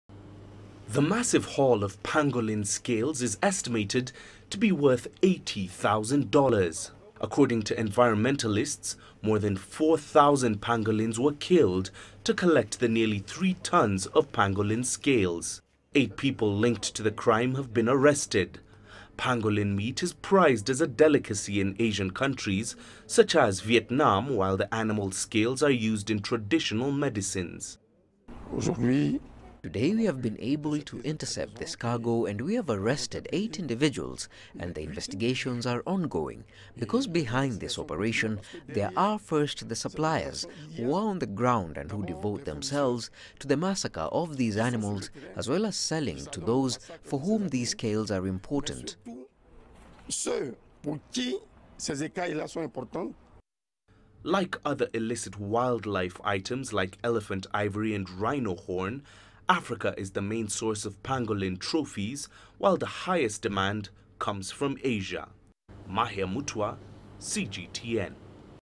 Authorities in Cote d'Ivoire seize huge haul of pangolin scales
Ivorian authorities have seized nearly 3 tonnes of pangolin scales - making it the biggest seizure of its kind. The trade of highly endangered pangolins was banned by the United Nations last year. Here's more on that story.